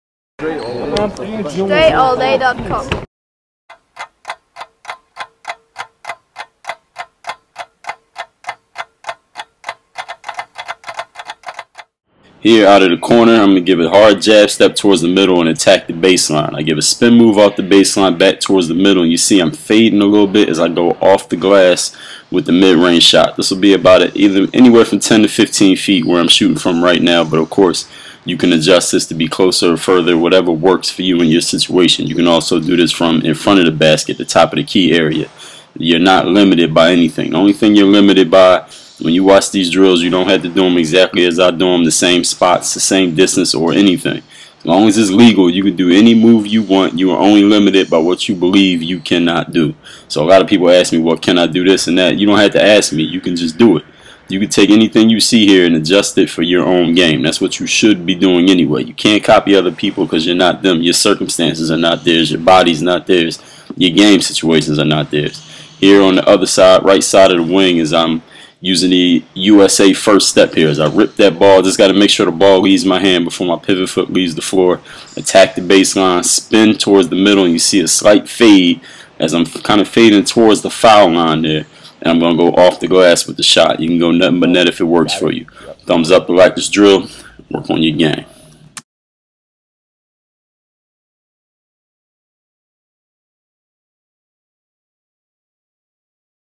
Kobe Bryant Baseline Drive, Spin Move Short-Fading Bank Shots | Dre Baldwin
Dre Baldwin talks about Kobe Bryant Baseline Drive, Spin Move Short-Fading Bank Shots.